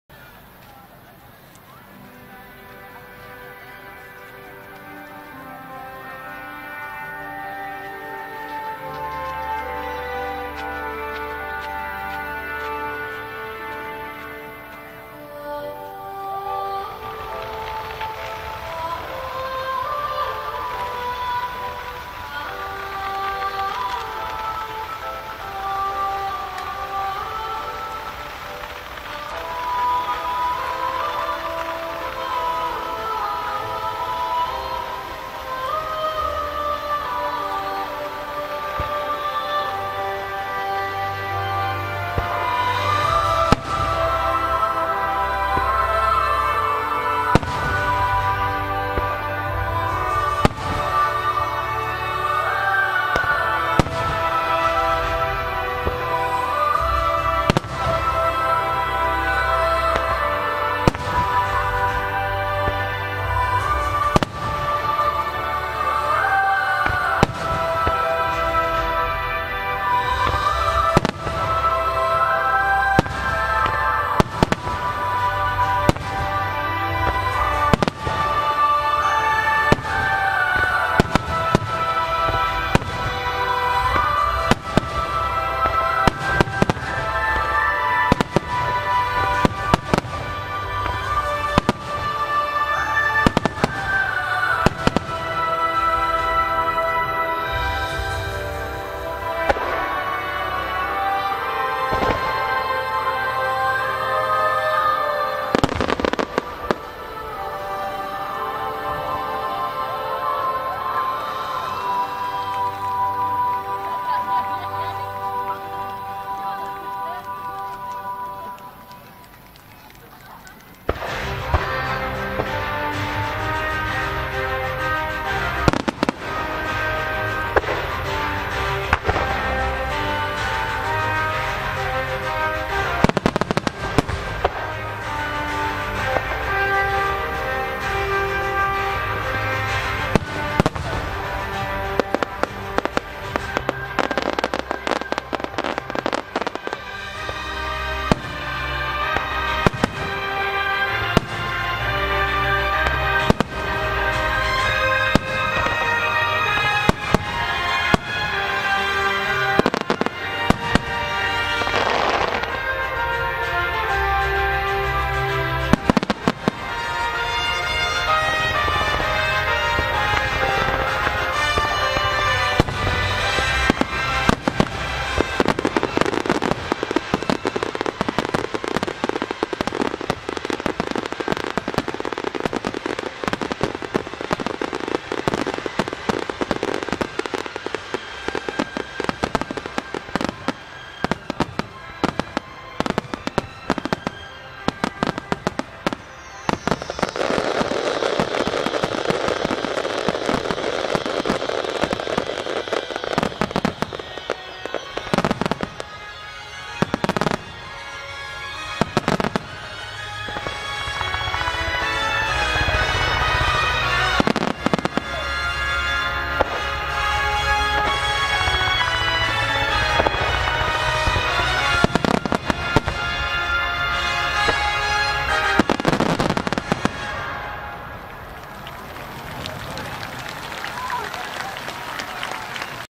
[4K]いせはら芸術花火大会2019 グランドフィナーレ メロデイー花火Grand Final Music Starmine
令和元年５月２５日　神奈川県伊勢原市総合運動公園で開催。
General movement park Isehara-city, Kanagawa Prefecture,Japan.
Date: 2019 May 25 (Saturday) 19:00 〜 19:55

花火師は（(株)磯谷煙火店　Fireworks manufacturer is Isogai  Fireworks co.ltd.

この花火大会は自治会をはじめ地域の方々の協力を得て市民により開催されます。
This fireworks display is held by citizens with the cooperation of local residents including the residents' association.
这个烟花汇演由市民在当地居民（包括居民协会）的合作下举行。
การแสดงดอกไม้ไฟนี้จัดขึ้นโดยประชาชนด้วยความร่วมมือของชาวท้องถิ่นรวมทั้งสมาคมผู้อยู่อาศัย